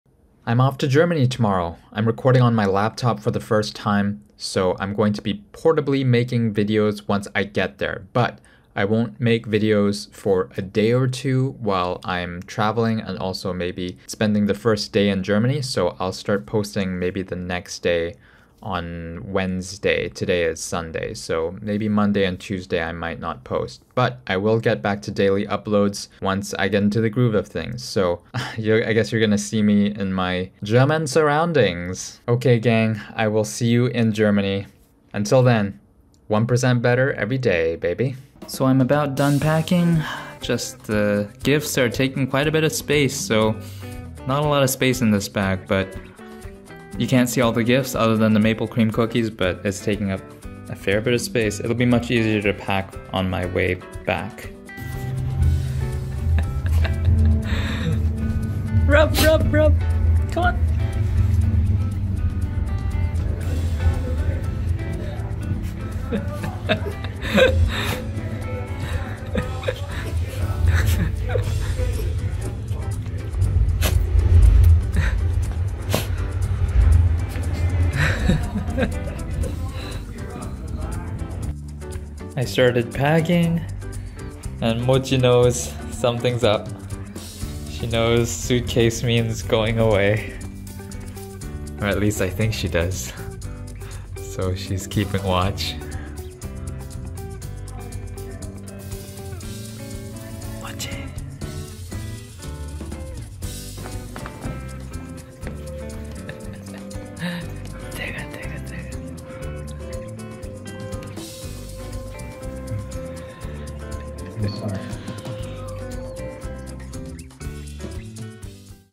I’m off to Germany tomorrow - 1% Better Day 147
First time missing my daily uploads…